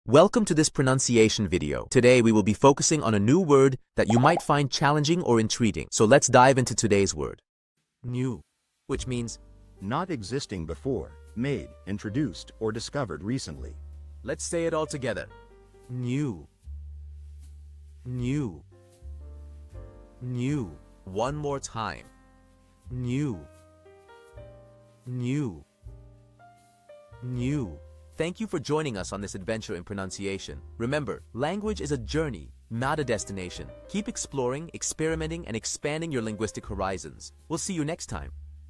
How to pronounce New
Master the Pronunciation of 'New Which Means New' - which means : Not Existing Before;Made, Introduced, or discovered recently. 📝 with @PizzaPronunciation 🎓🗣️ - [Your Guide to Italian]

Learn to pronounce 'New' perfectly in Italian with our easy and engaging tutorial by @PizzaPronunciation! 📖📽️ In this comprehensive video, we provide you with native speaker examples 🗣️👂 and phonetic breakdowns 🎵 to ensure you get the pronunciation just right.

How to say New in Italian